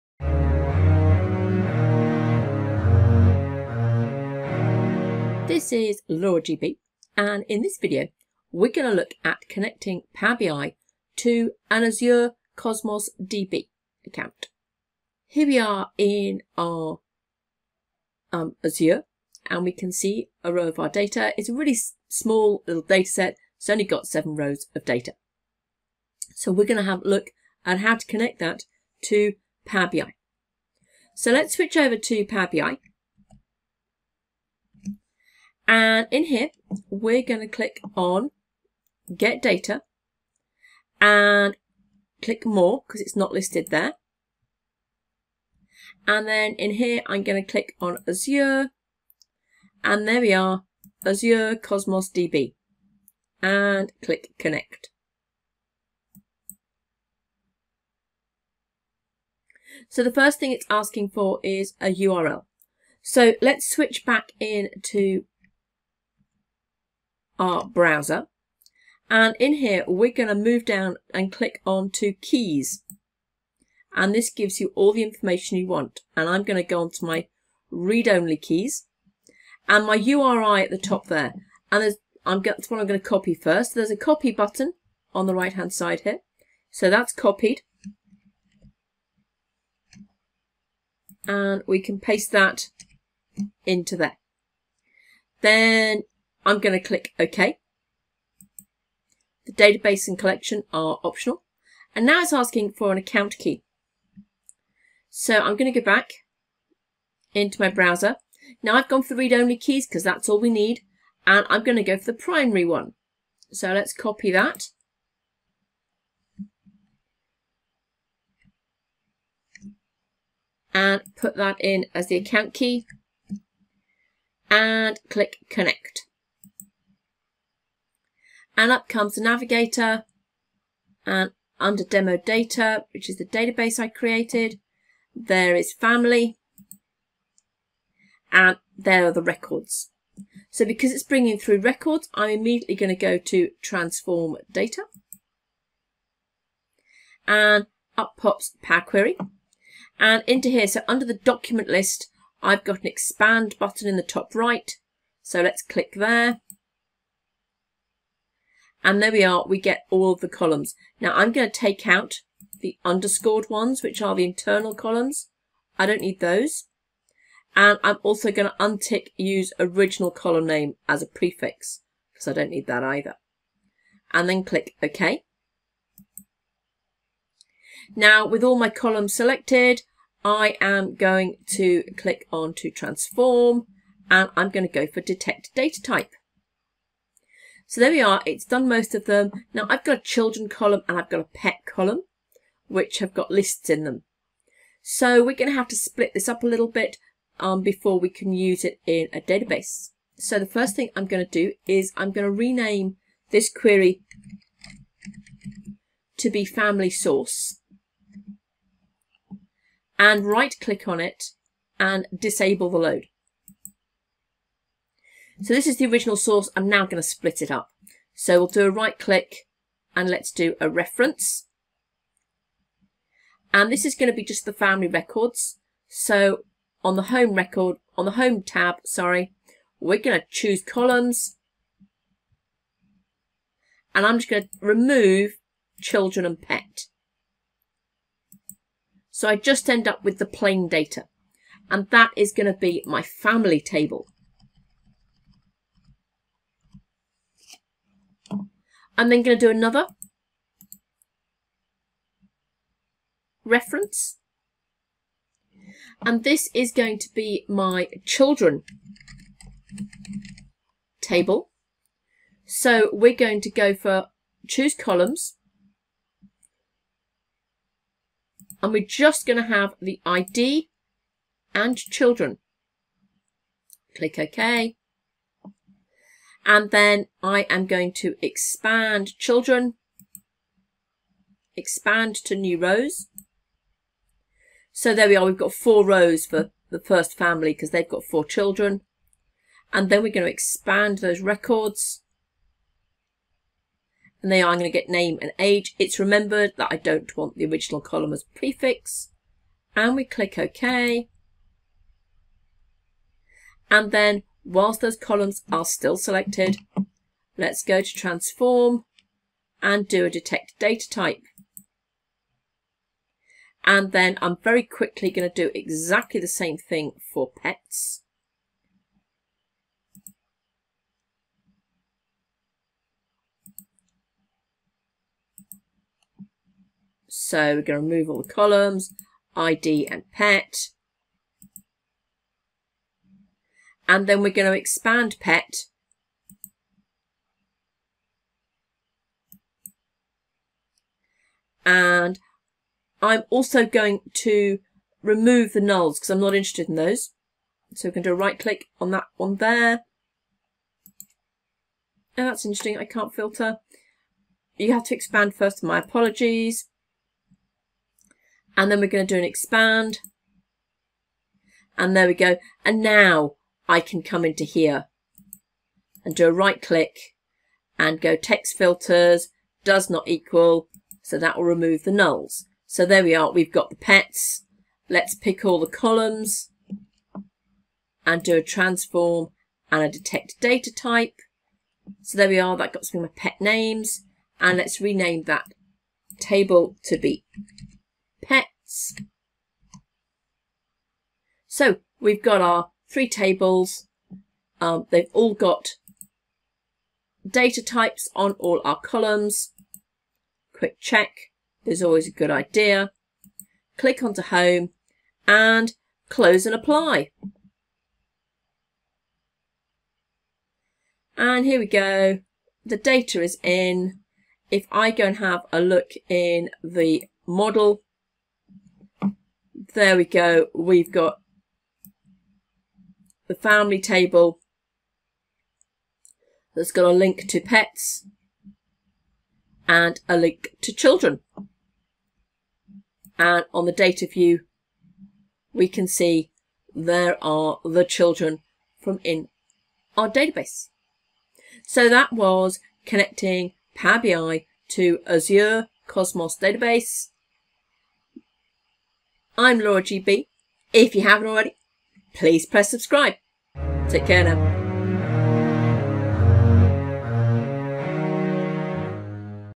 Power BI Connect to Azure Cosmos Db
This video covers connecting a Power BI report to Azure Cosmos Db and then splitting the nested tables into a simple star schema.

00:00 - Introduction
00:34 - Creating Power BI Connection
02:25 - Transform Records
03:11 - Split Nested Tables